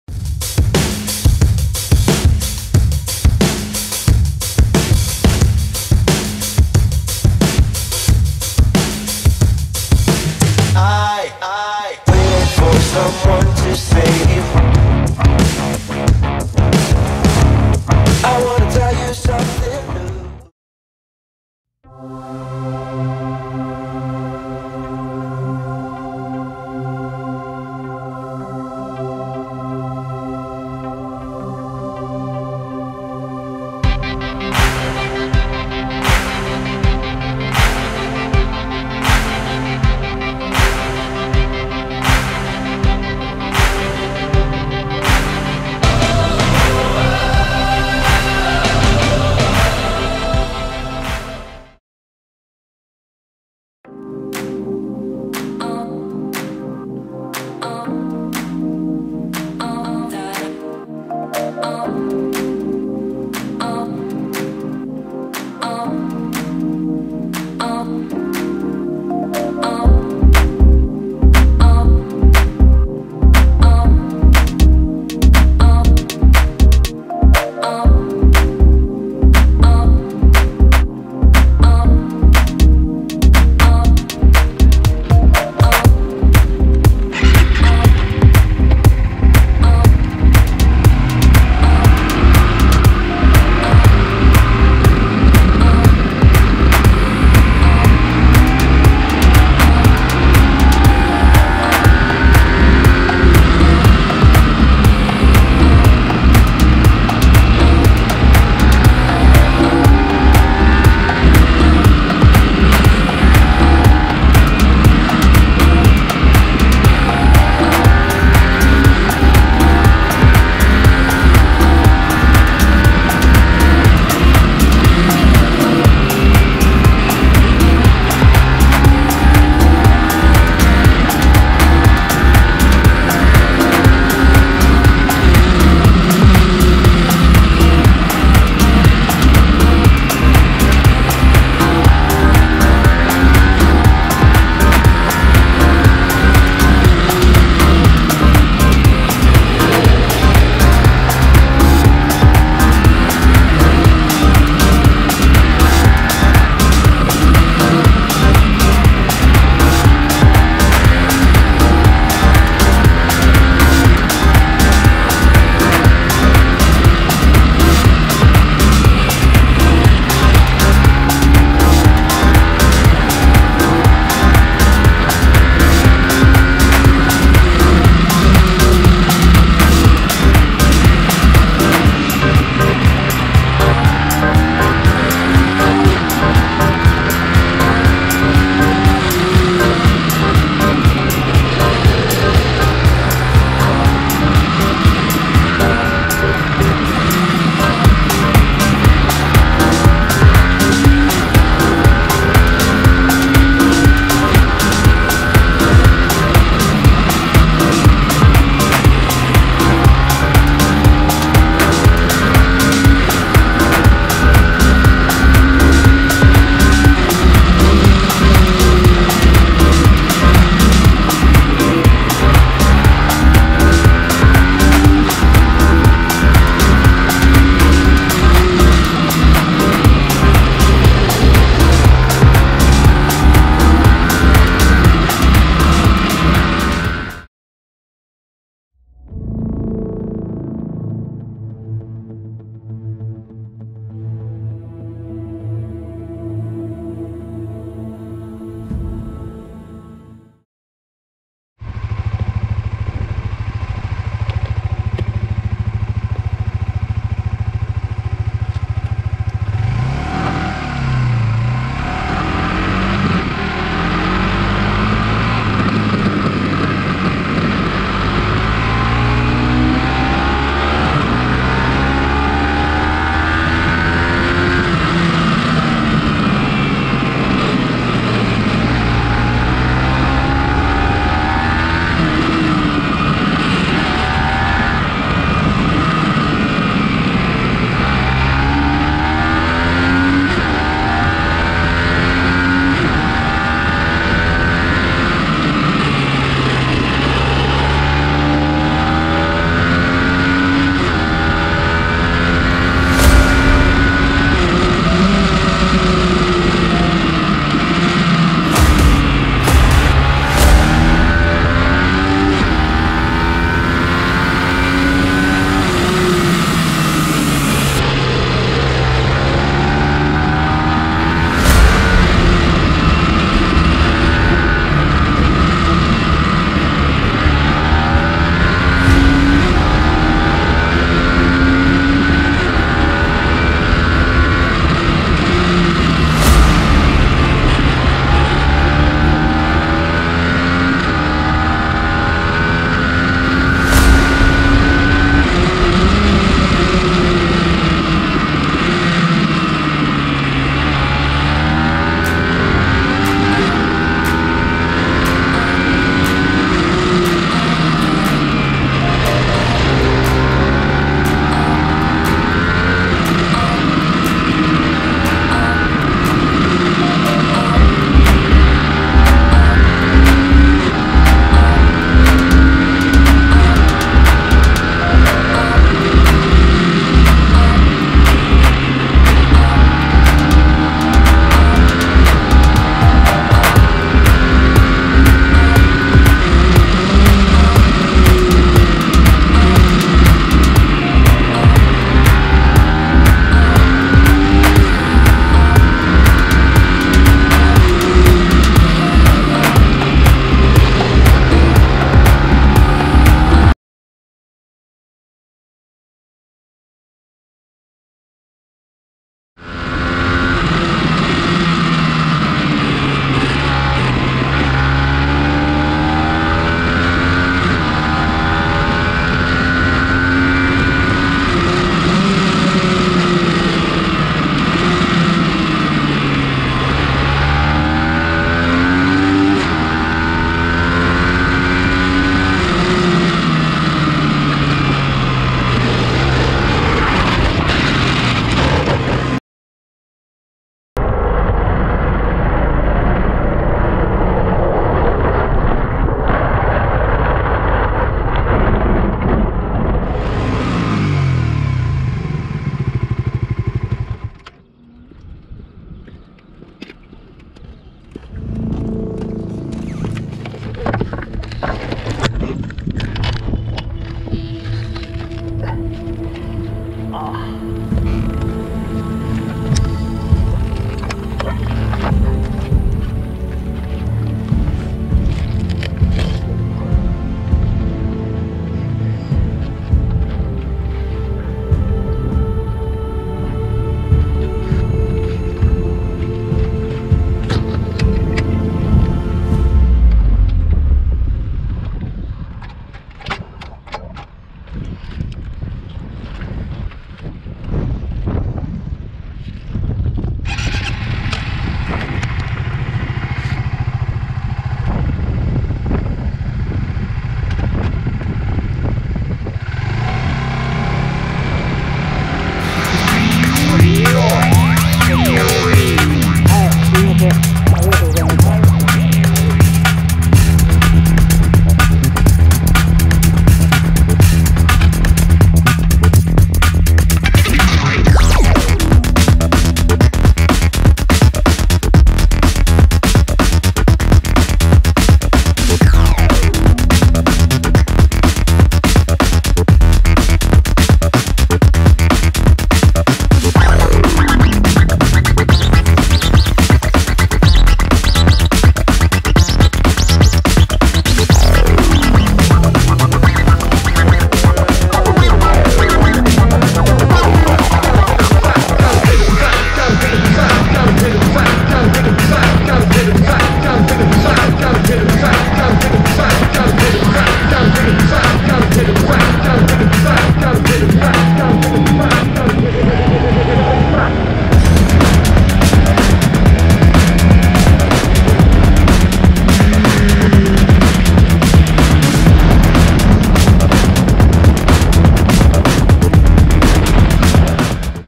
【GSX-R125】【アラ還リターンライダー】自己ベスト更新そして転倒
今シーズン初めからマフラーに穴があいていて排気漏れの状態で走っておりました。今回はそこを補修しての走行になります。